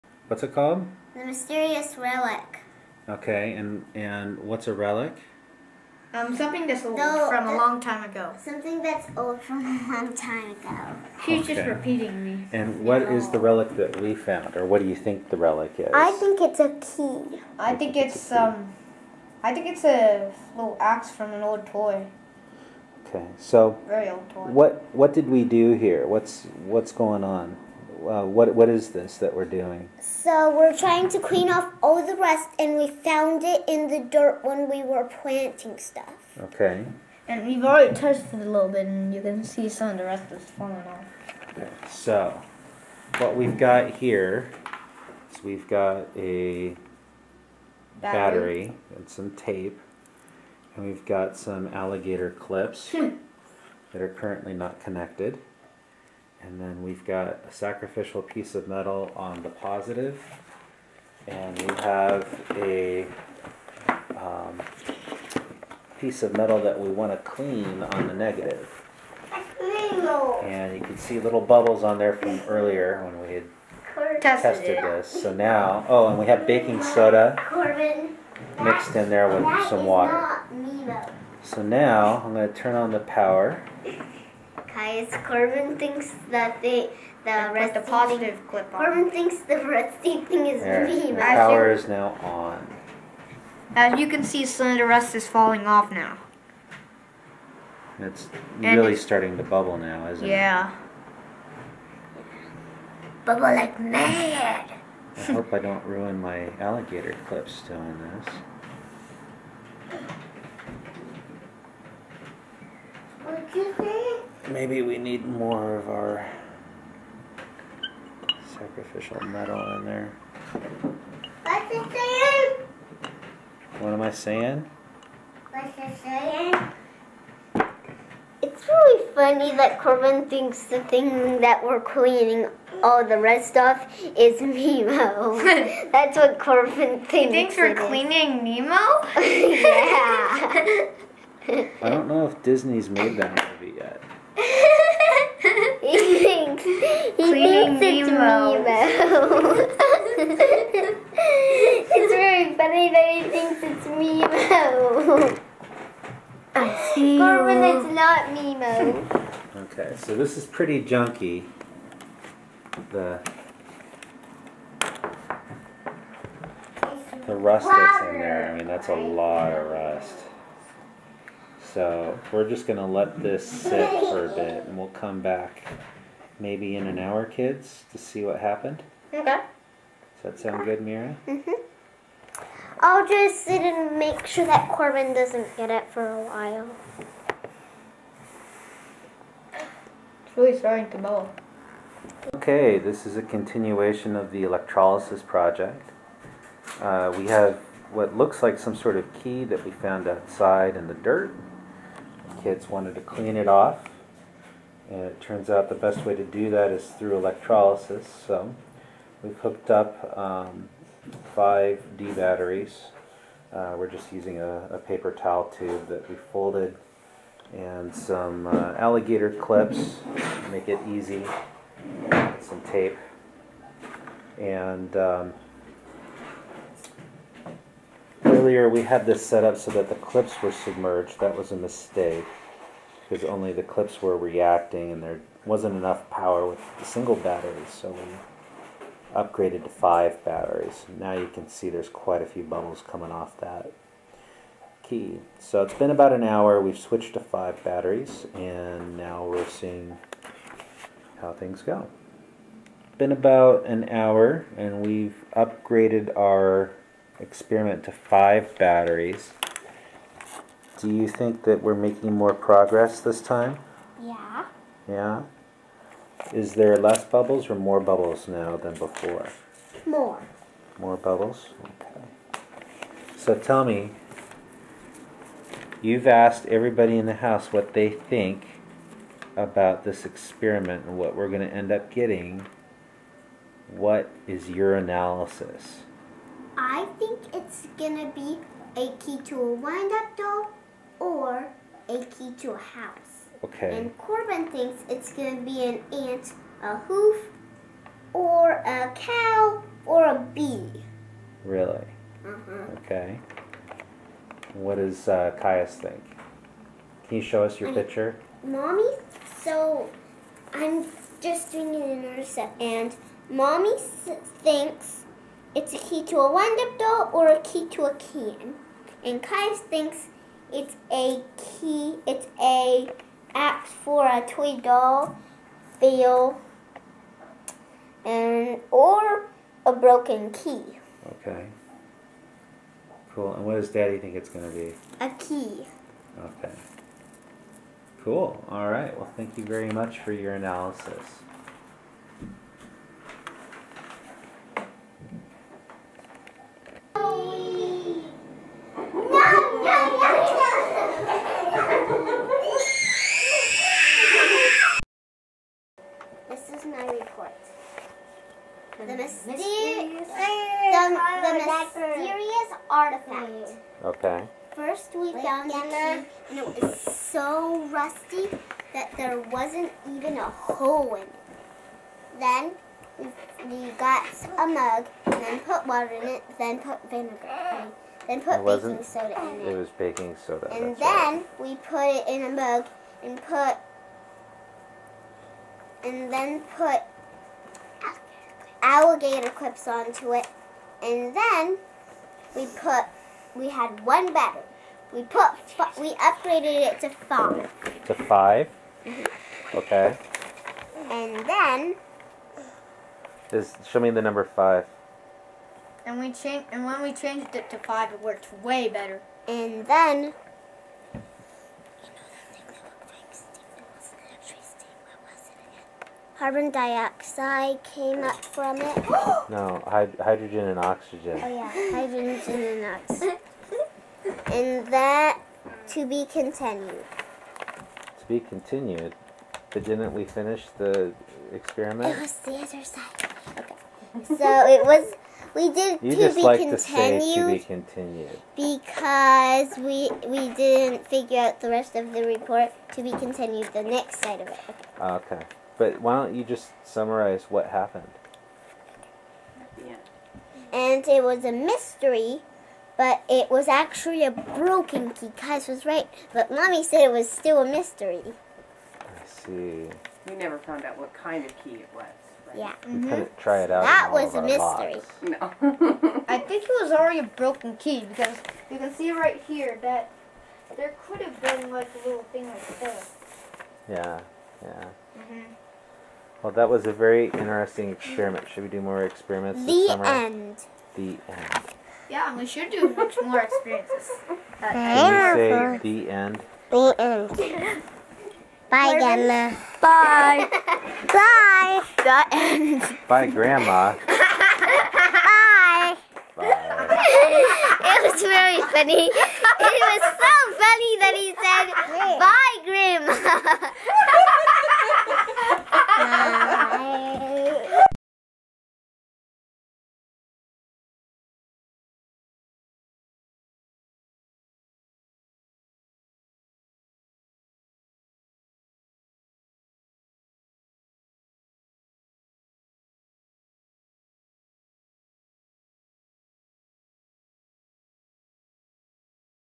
Kids Teach Dad About Electrolytic Rust Removal Using Household Materials
Kids use electrolysis to clean rust from an old iron key using materials found about the house.

Materials: Glass mug, tap water, baking soda, alligator clips, batteries, tape, and some sacrificial (junk) metal.